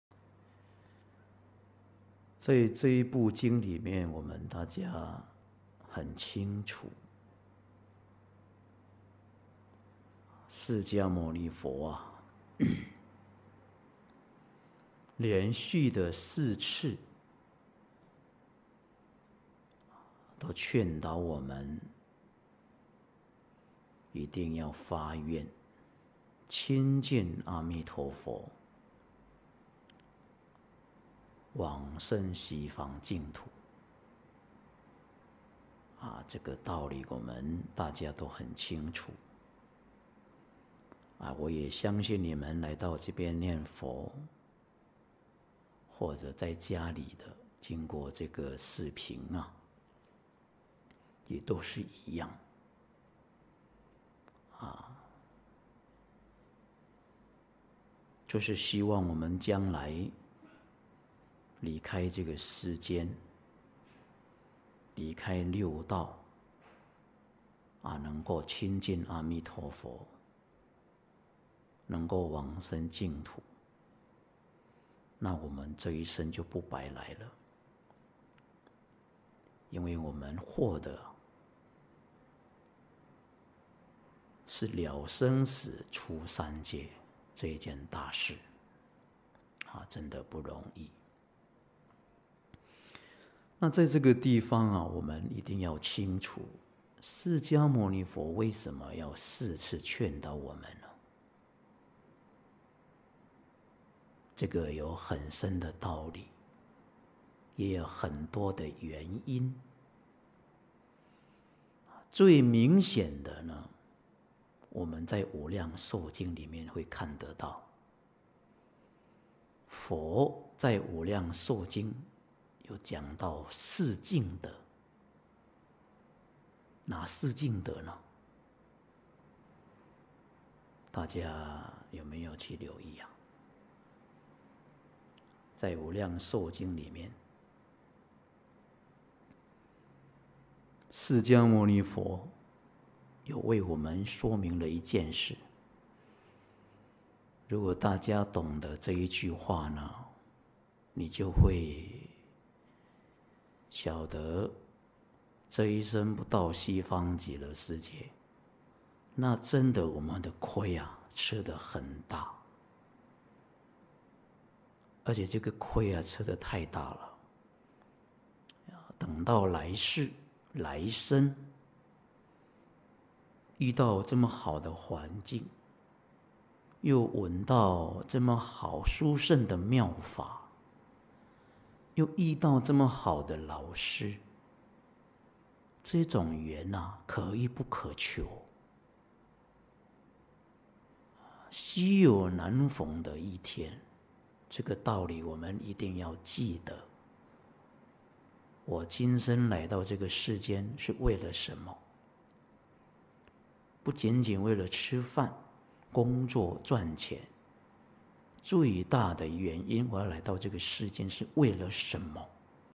學悟法師（044）a遇到這麽好的環境 又聞到這麽好 殊勝的妙法 又遇到這麽好的老師 這種緣 可遇不可求 稀有難逢的一天 這一生不到西方極樂世界 這個虧吃的太大了 202135
在這一部經裡面我們大家很清楚 釋迦牟尼佛 連續的四次 都勸導我們一定要發願 親近阿彌陀佛 往生西方淨土 這個道理我們大家都很清楚 我也相信你們來到這邊 念佛 或者在家裡 經過這個視頻 也都是一樣 就是希望我們將來 離開這個世間 離開六道 能夠親近阿彌陀佛 能夠往生淨土 那我們這一生就不白來了 因爲我們獲得 是了生死出三界 這一件大事 真的不容易
 在這個地方我們一定要清楚 釋迦牟尼佛爲什麽要 四次勸導我們呢 這個有很深的道理 也有很多的原因最明顯的 我們在《無量壽經》裡面 會看得到 佛在《無量壽經》有講到四淨德 哪四淨德呢 大家有沒有去留意 在《無量壽經》裡面 釋迦牟尼佛有爲我們說明了一件事 如果大家懂得這一句話呢 你就會曉得這一生不到西方極樂世界 那真的我們的虧 吃的很大 而且這個虧吃的太大了！等到來世 來生 遇到這麽好的環境 又聞到這麽好 殊勝的妙法 又遇到這麽好的老師 這種緣 可遇不可求 稀有難逢的一天 這個道理我們一定要記得
 我今生來到這個世間是爲了什麽 不僅僅爲了吃飯 工作賺錢 最大的原因 我來到這個世間是爲了什麽